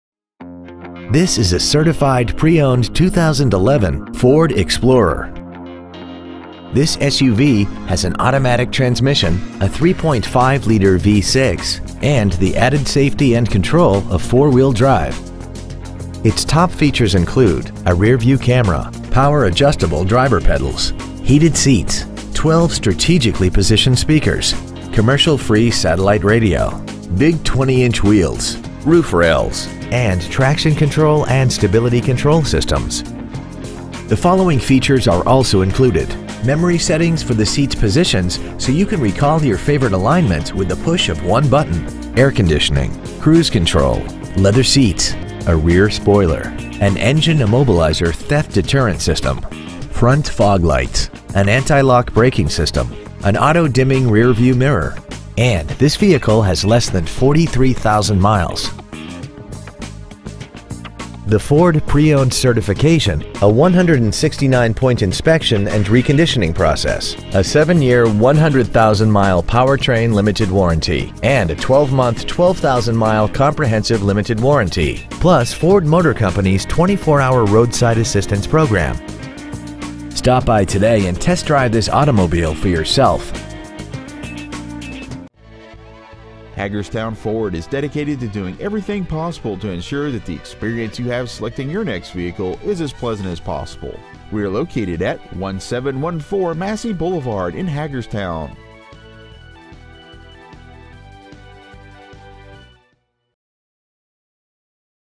2011 FORD EXPLORER Certified Hagerstown MD
We have been honored to serve the Hagerstown MD area , we promise that your experience at our dealership will exceed your expectations !

 Year : 2011
 Make : FORD
 Model : EXPLORER
 Engine : 3.5L V6 24V MPFI DOHC
 Trans . : 6-SPEED AUTOMATIC
 Exterior : KONA BLUE METALLIC
 Miles : 42,931
 Interior : BLACK
 Stock : R9803

 1714 Massey Blvd.